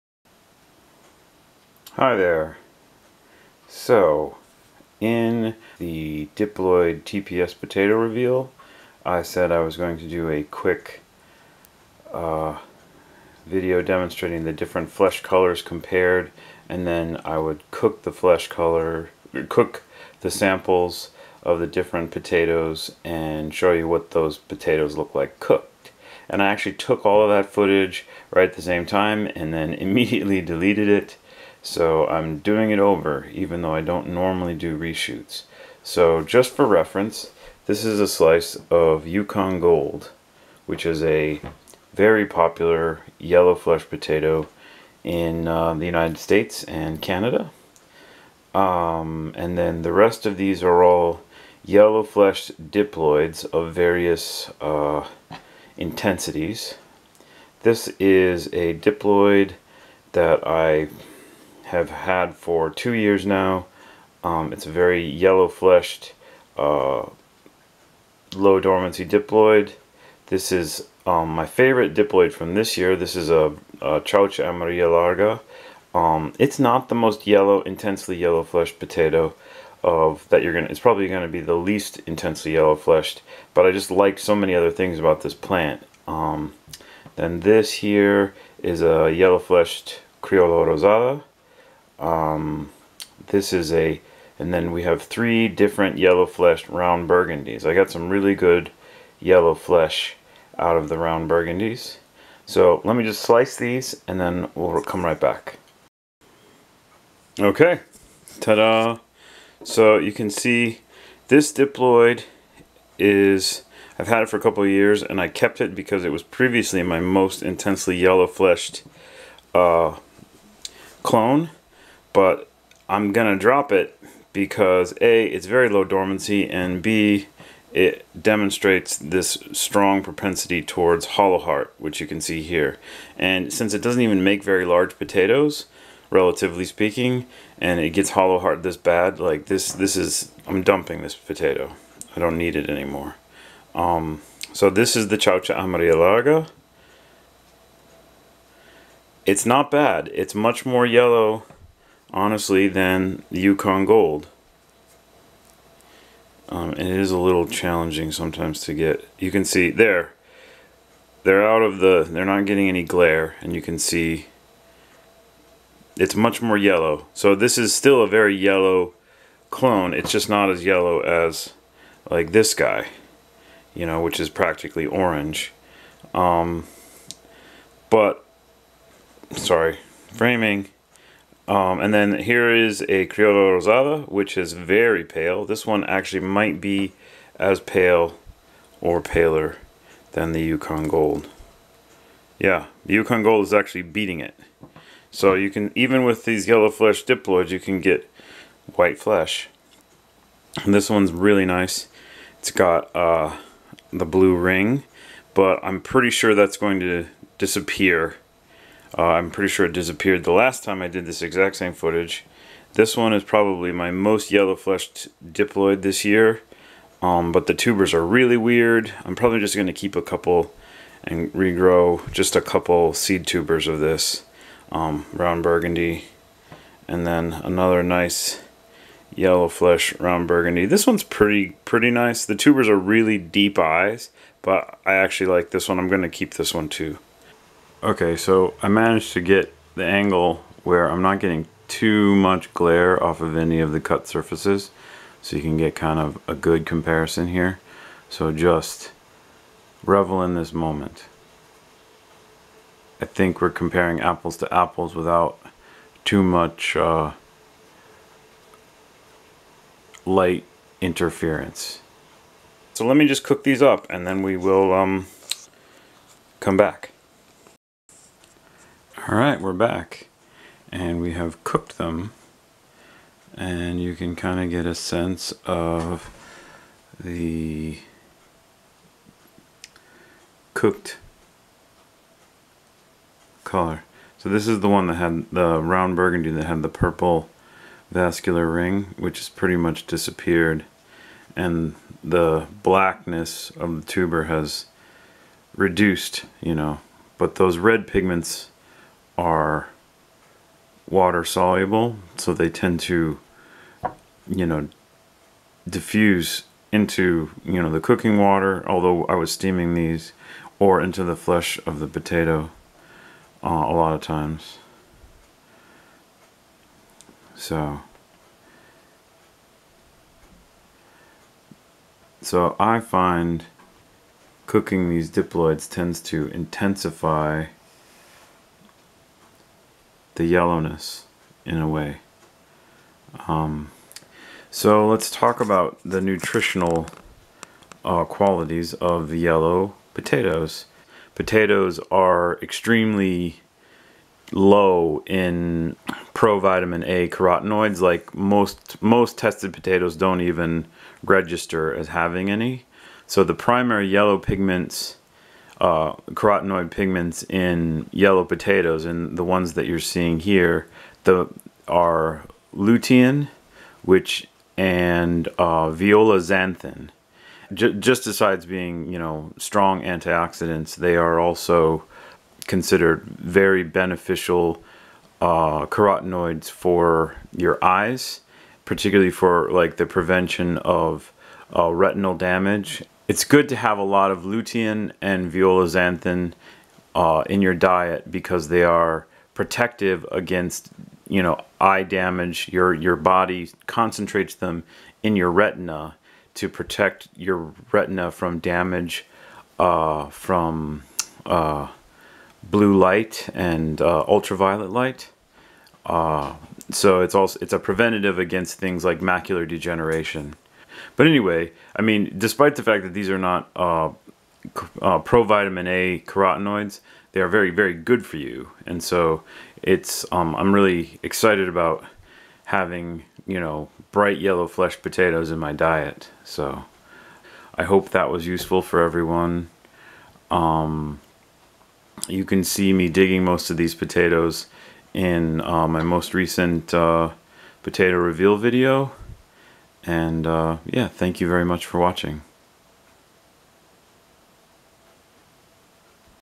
Yellow Flesh Potato Smackdown! Comparing Yellow Flesh Diploids with Yukon Gold
After my most recent potato reveal video digging some diploid potatoes, I stated I would make a video comparing their uncooked and cooked colors alongside Yukon Gold.  Yukon Gold is a very popular and common tetraploid potato in the USA and Canada, and so it makes a good reference potato with yellow flesh for comparison to these diploids.

It is very common for phureja type diploid potatoes to have very brightly colored skin, and often for the flesh colors to be intensely pigmented with yellow, red, or purple pigments.  The yellow carotenoid pigment levels in these diploids can range almost 22 times higher than in standard tetraploid potatoes like Yukon Gold.  I am happy to see that some of the diploid potato seedlings I am growing this year are so much more intensely yellow than plain old Yukons.

Although these yellow carotenoid pigments in potatoes are very close structurally to Beta Carotene and other provitamin A carotenes, the human body cannot convert them into Vitamin A.  They are however very powerful antioxidants, and the two primary potato carotenes- Lutein and Violaxanthin- are very important pigments for the protection of the retinal macula from damage from ultraviolet and blue light.  So these pigments are an extremely healthy addition to the diet for a variety of reasons.